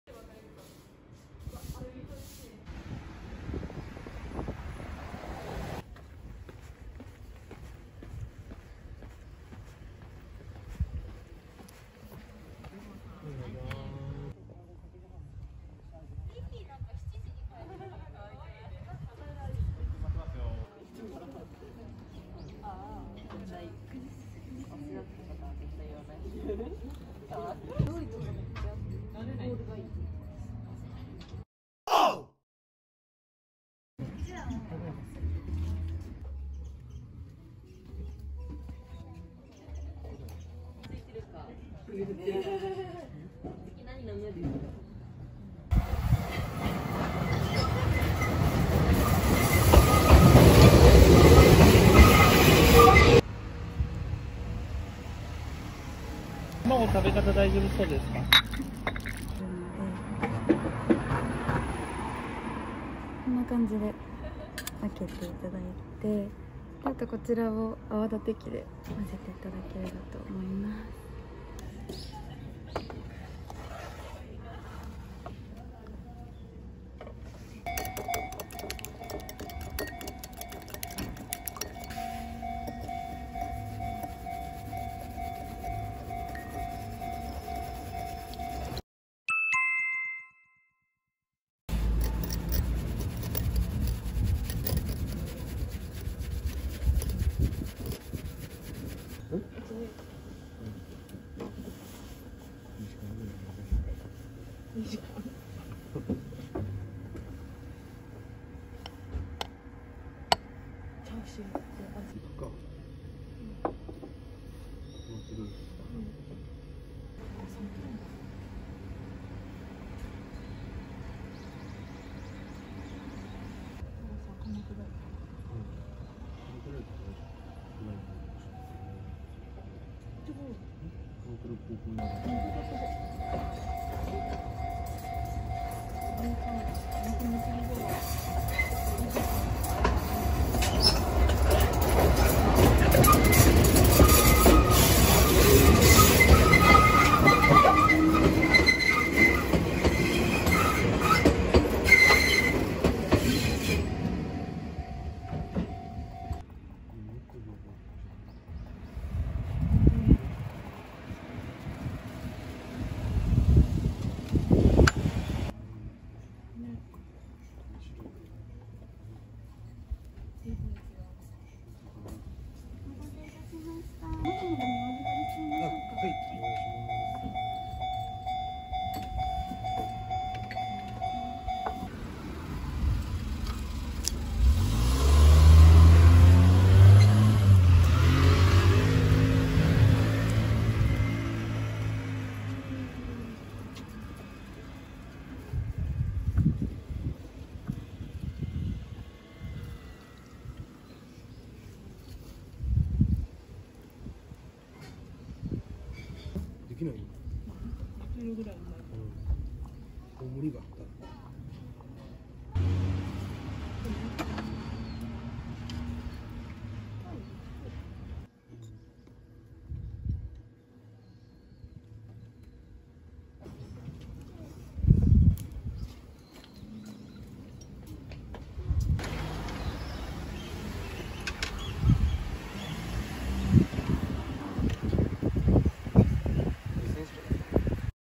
Raw Egg Over Rice & Local Train!! Japanese Breakfast - YORIDOKORO -
“Yoridokoro” is popular Japanese restaurant in Kamakura-shi, Kanagawa. It’s famous for raw egg over rice, dried fish and local train “Enoden Line”. Raw egg is safe in Japan and it’s called Tamago Kake Gohan（TKG）in Japanese.